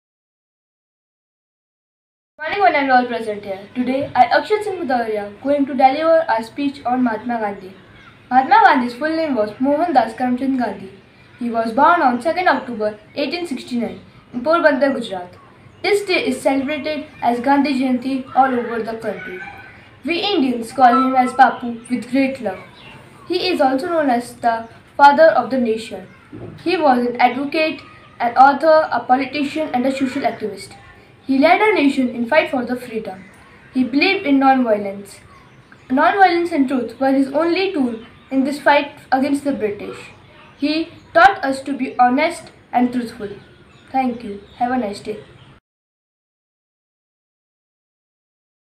Gandhi jayanti/ Gandhiji/ 10 lines on Mahatma Gandhi in English Essay /English essay// Short essay
Happy Gandhi Jayanti.Let’s celebrate peace, freedom and non-violence and join hands to build a better India.